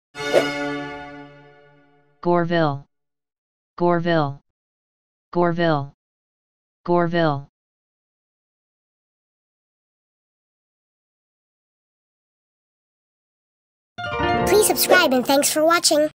Goreville (How to Pronounce Cities of the World)💬⭐🌍✅
This video is about "Goreville". This video series is something special. We're covering the pronunciation of all the cities of the world.